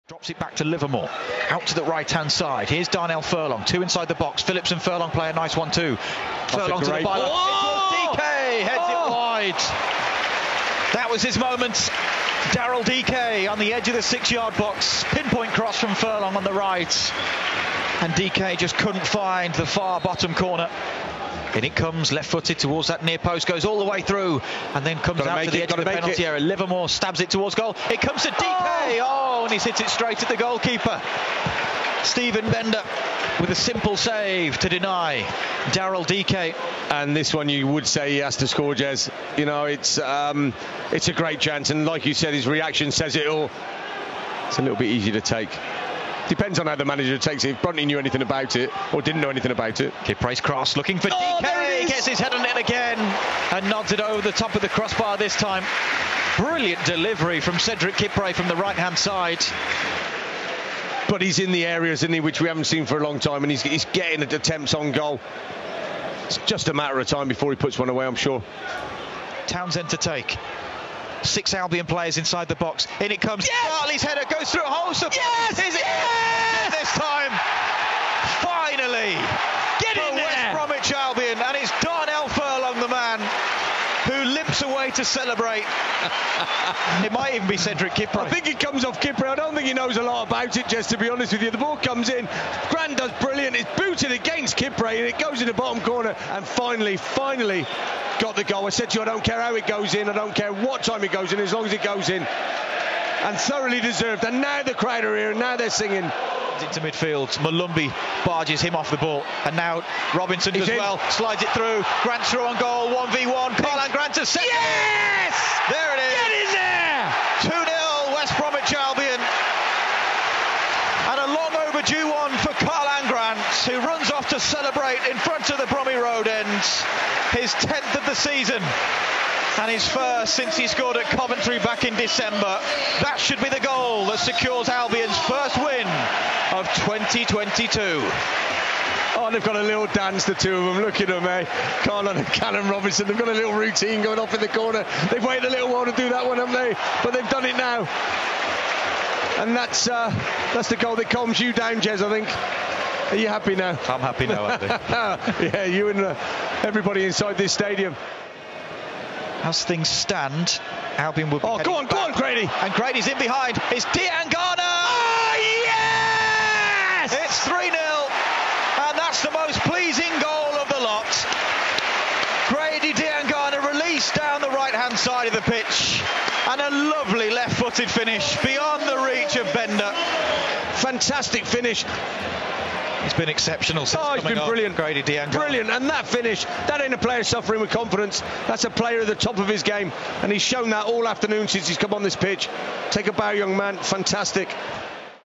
West Bromwich Albion v Peterborough United highlights
Watch full highlights from Albion's first win of 2022.